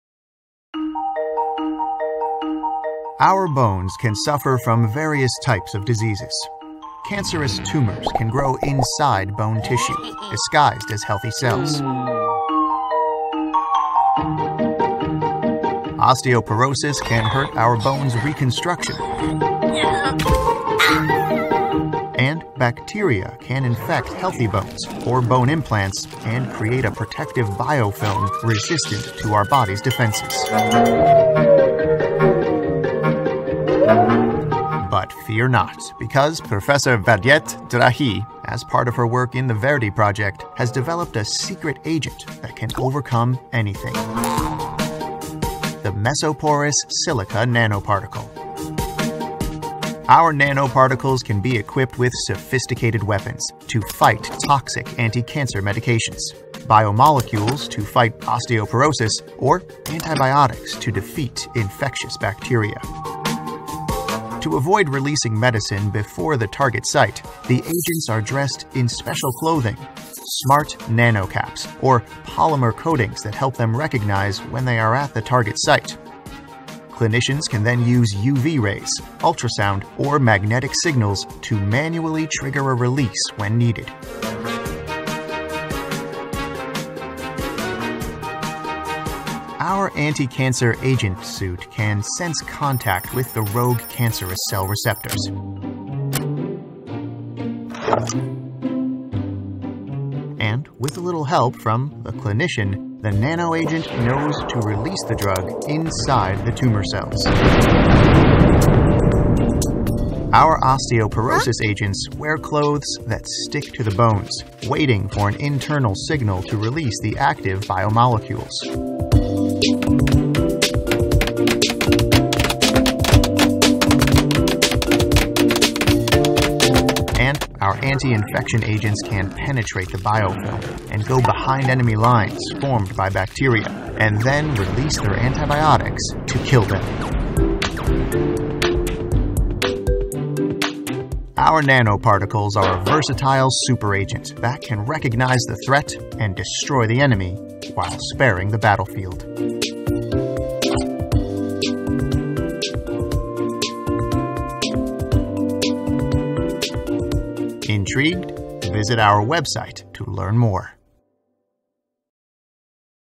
Nano medicina española para huesos heridos
María Vallet-Regi explica el desarrollo de nanoesferas de sílice para llevar fármacos antitumorales hasta los huesos.